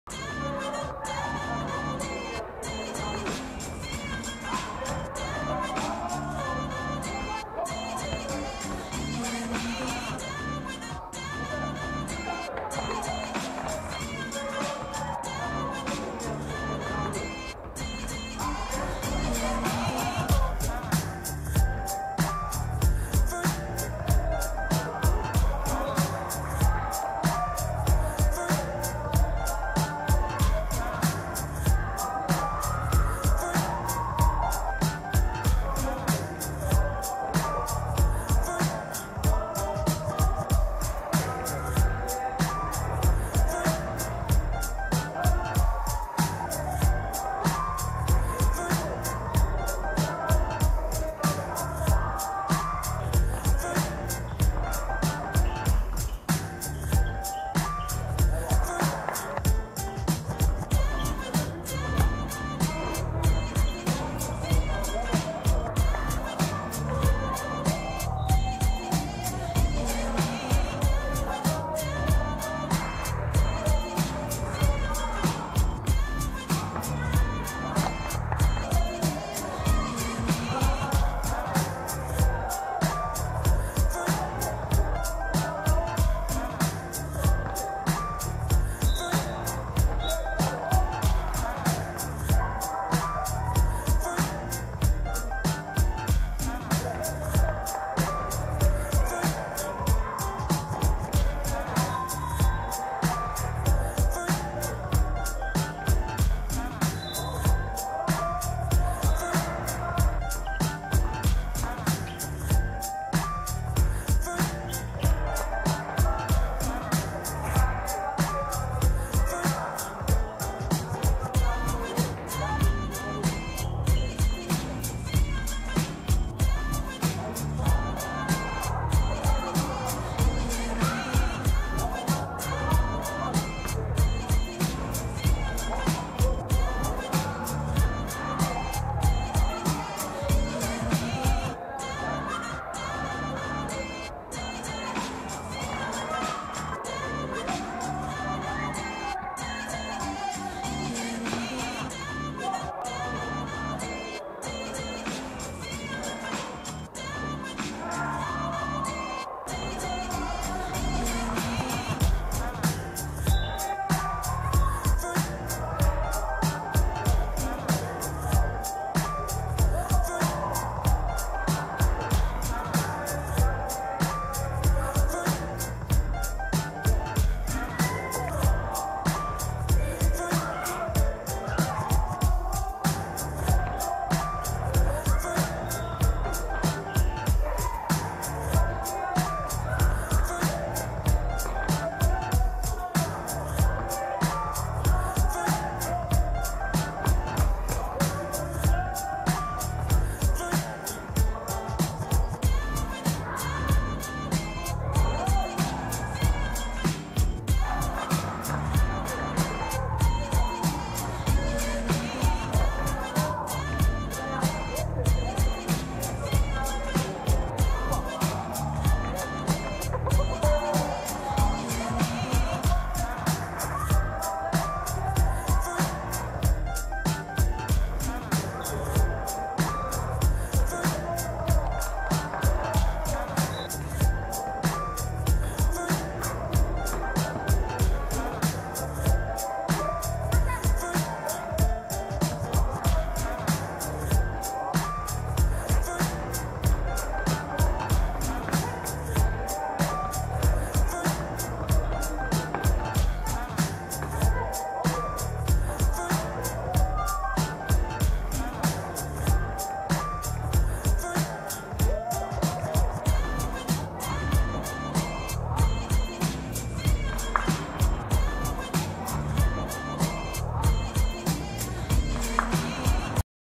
23 02 Kangury vs Bizony – Dzika Liga Junior 8
KANGURY 33:65 BIZONY
N.Hajewski 24pkt 10zb 6ast 6stl
M.Pietrasiewicz 17pkt 20zb